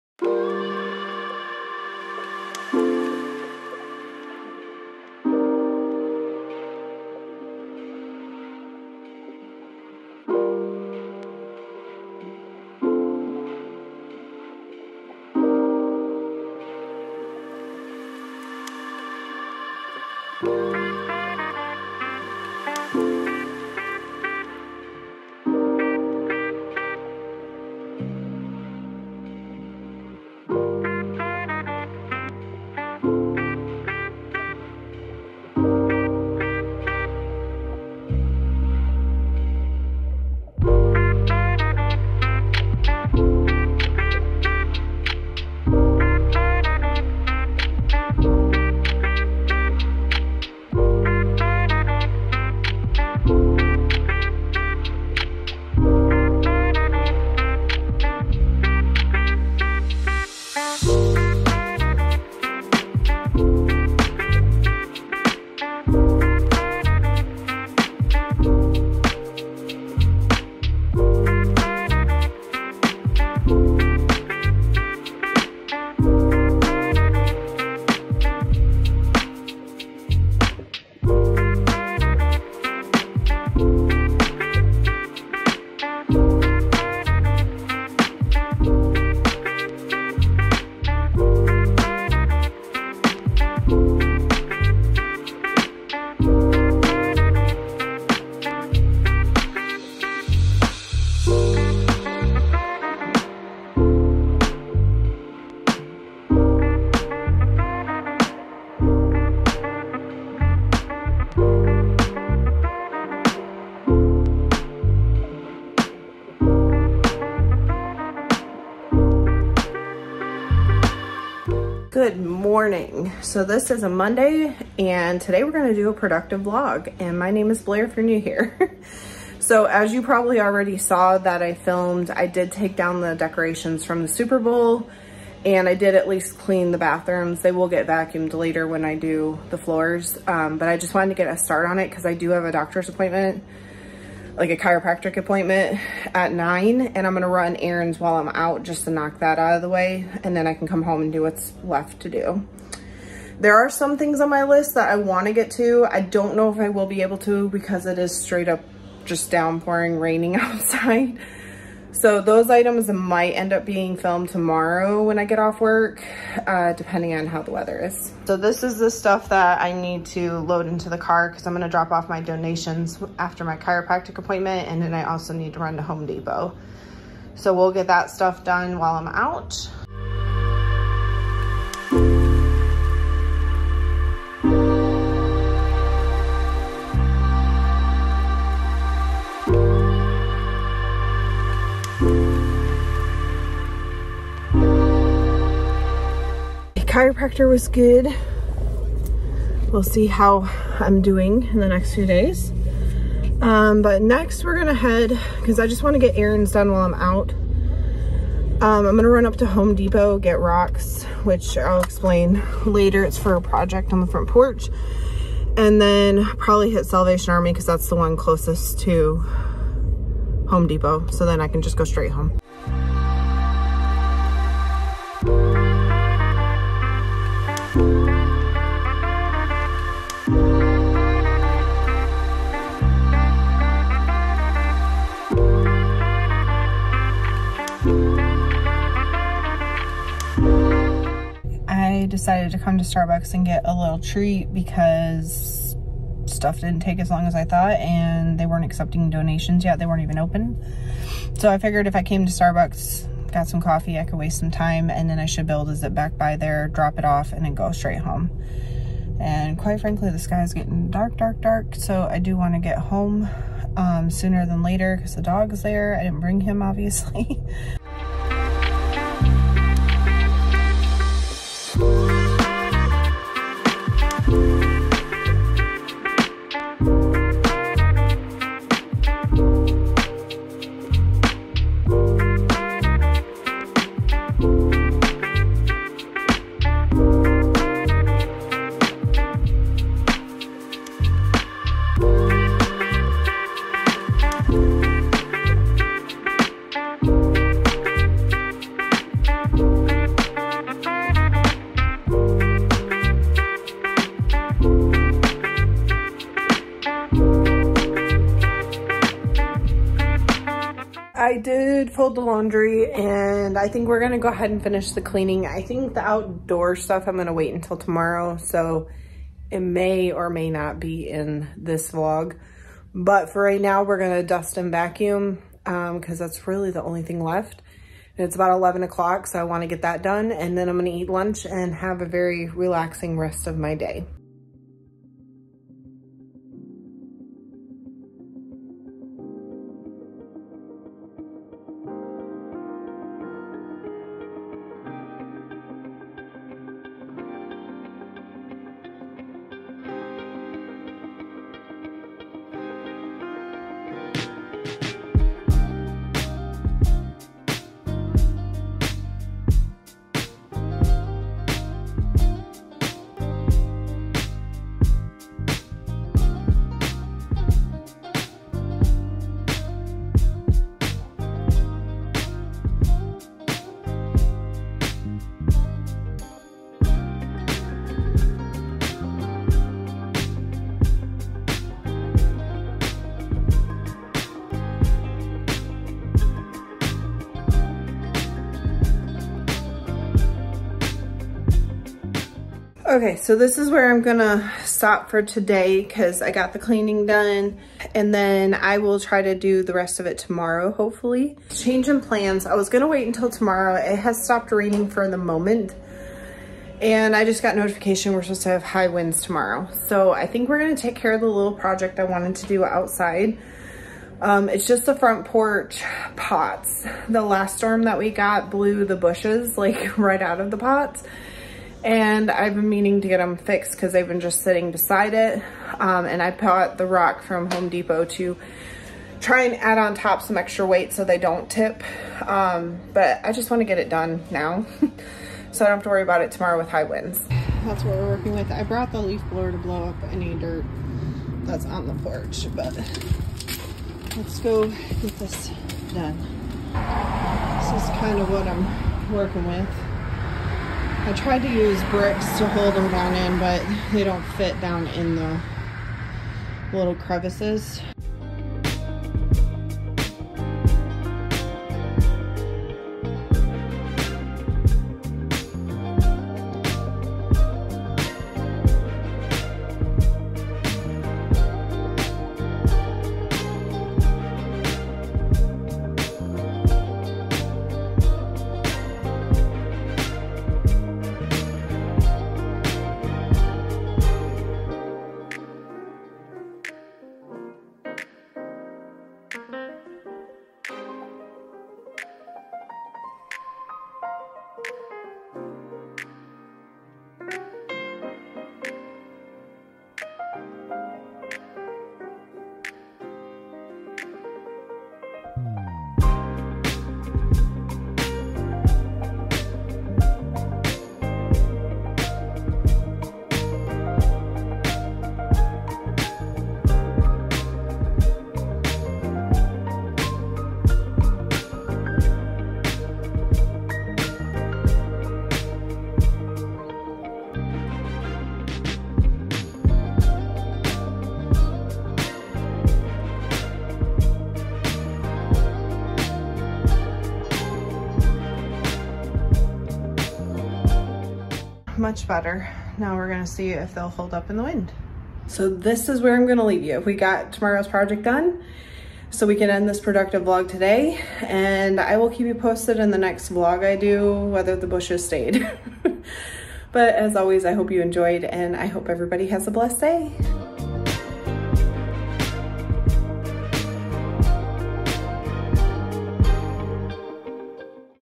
Productive Vlog | Cleaning, Errands and a Fix it Project
Today’s going to be productive! Running Errands, cleaning the house and Fix it project. I hope you enjoy!